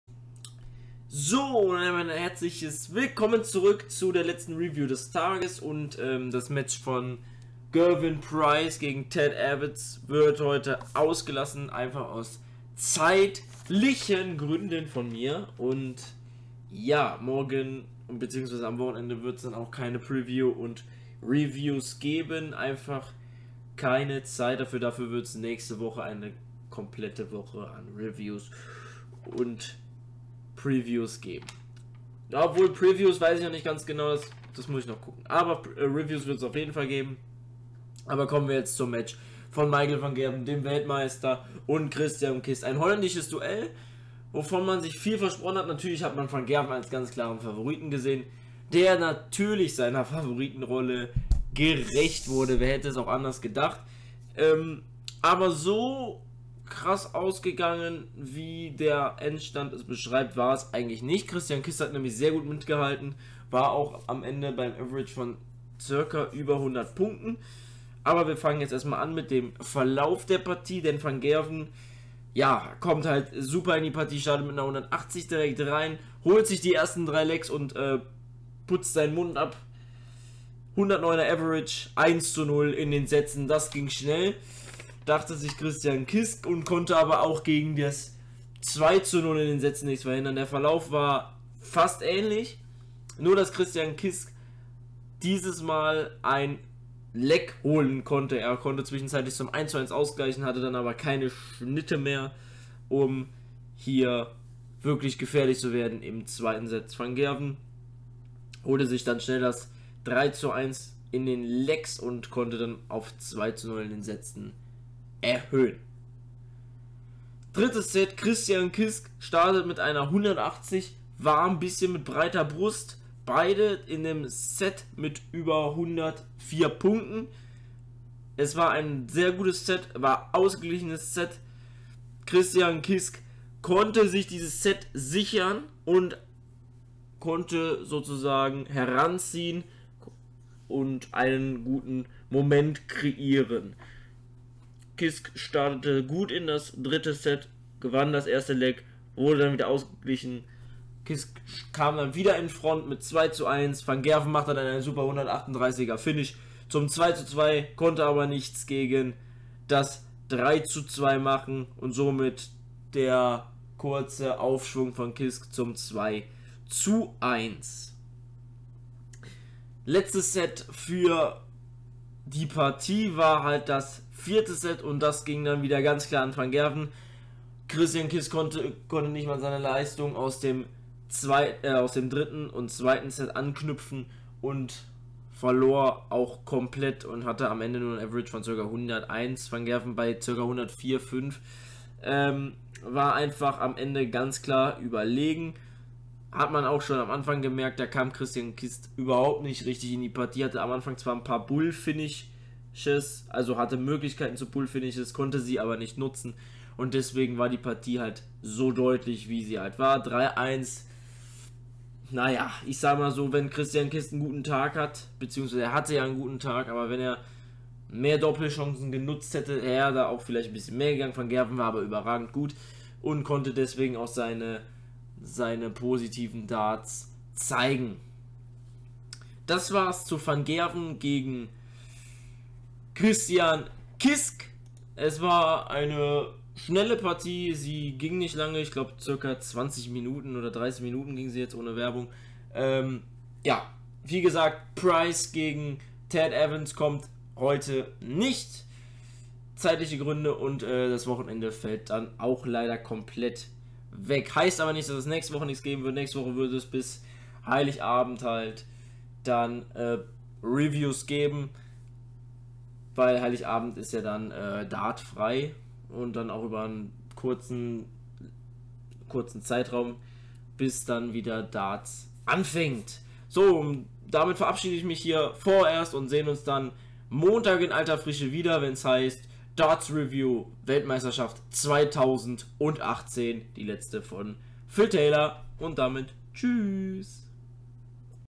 Van Gerwen vs Christian Kist Darts Wm / Review
Letzte Partie für mich heute über die ich reden werden. (zeitliche Gründe)

Wer gewinnt?
Wie ist der Verlauf?

Sehen sie jetzt kurz und knackig in dieser Review